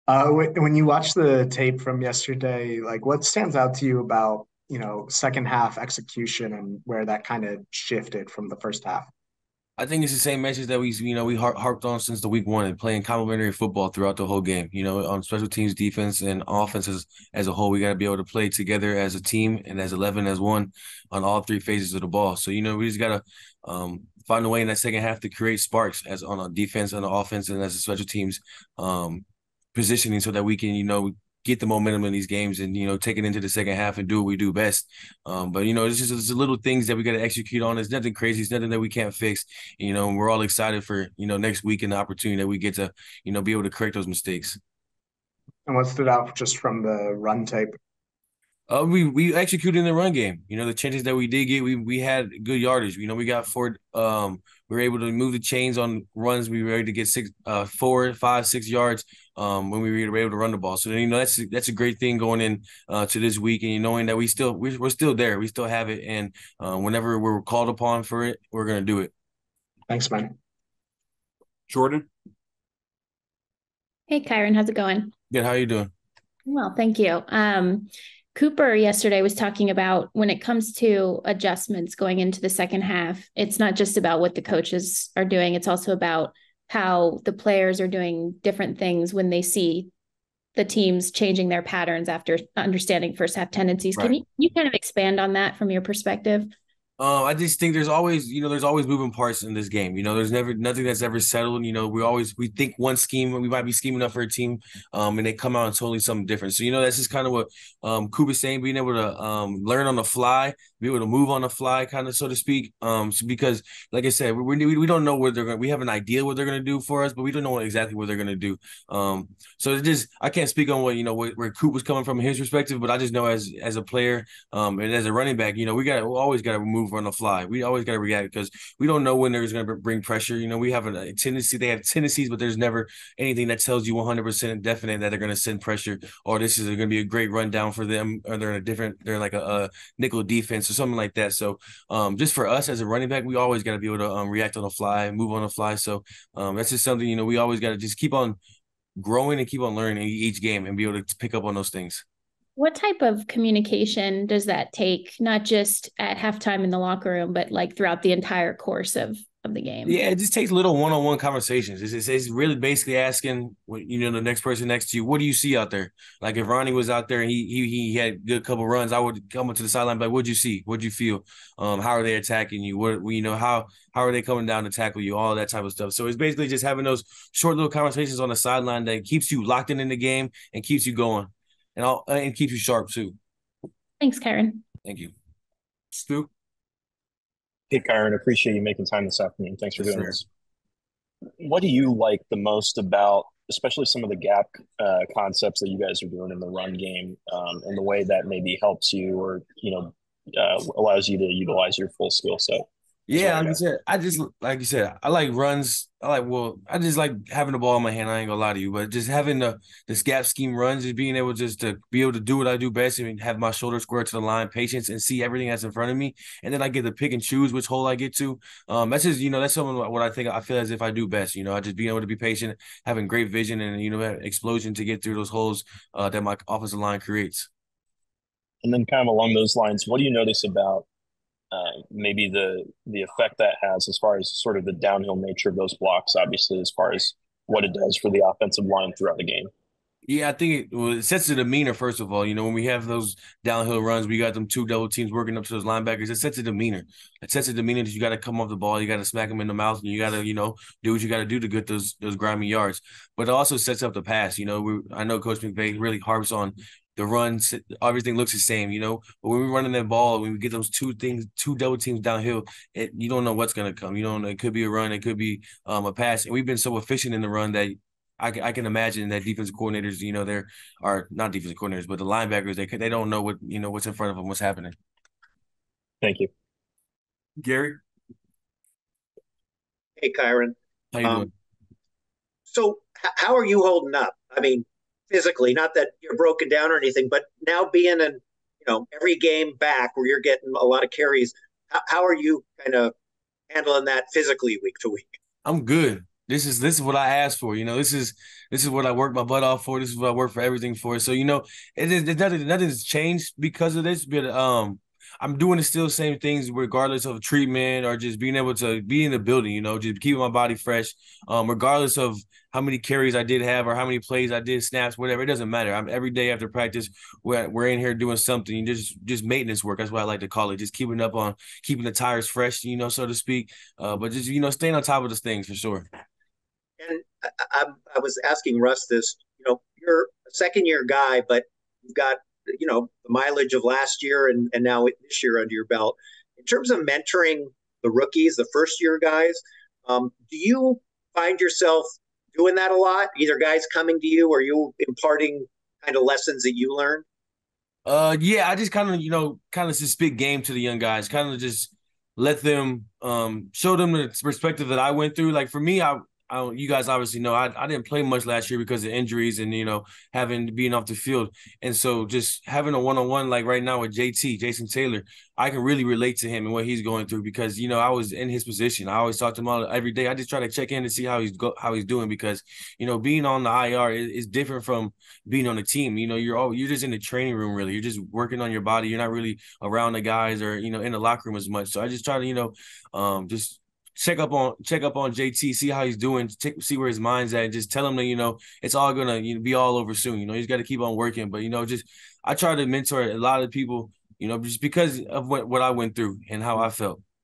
Kyren Williams Talks Takeaways From Rams Run Game In Week 5 Against The Eagles
Los Angeles Rams running back Kyren Williams talks about what he saw from the team's run game in Sunday's game against the Philadelphia Eagles.